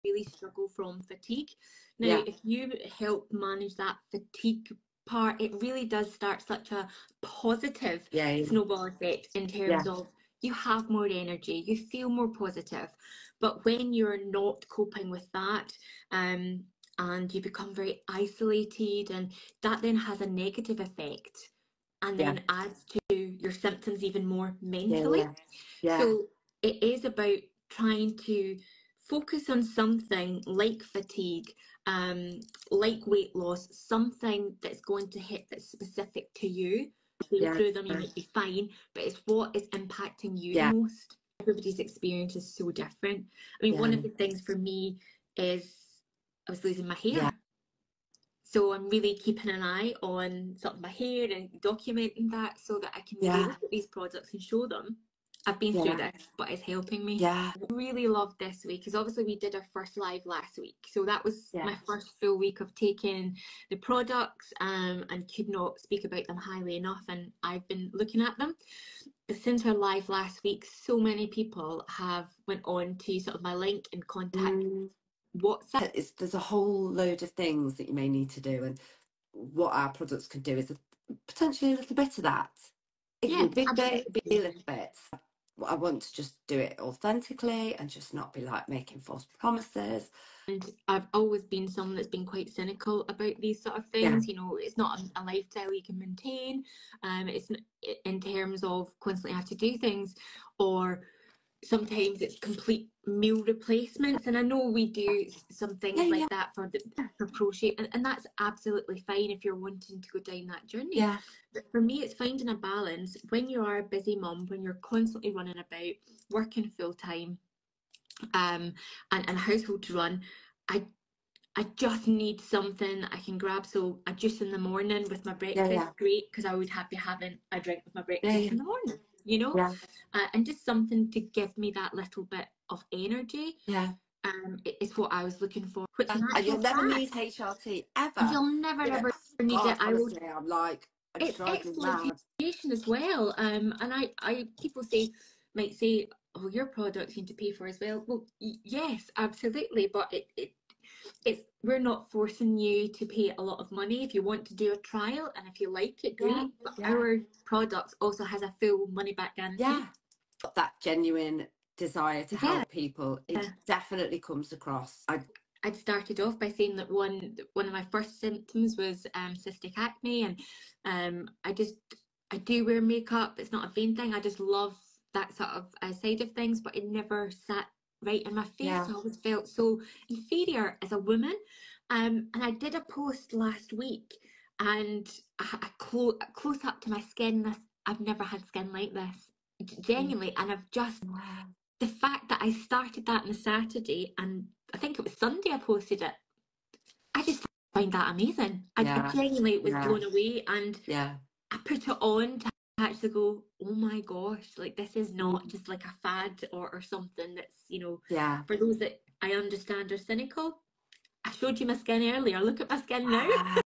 Perimenopause symptoms of fatigue, hair loss, weight gain and the impact on our skin
Perimenopause symptoms of fatigue, hair loss, weight gain and the impact on our skin can really have a negative effect on our daily lives and how we feel. @Jen Watson and myself chat about her first week using the products we both share and how they have helped her already 🙌🏻🥰